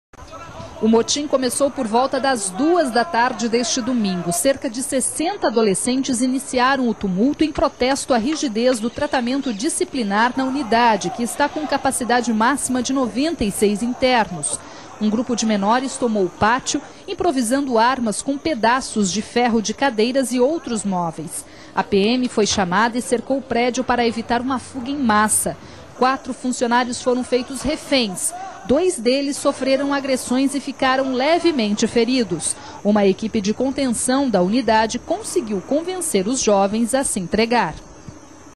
REBELIÃO FUNDAÇÃO CASA - 12/10/2009
DUROU CERCA DE CINCO HORAS O MOTIM DOS INTERNOS DA FUNDAÇÃO CASA DE SÃO JOSÉ DOS CAMPOS. DOIS FUNCIONÁRIOS SOFRERAM FERIMENTOS LEVES DURANTE O TUMULTO.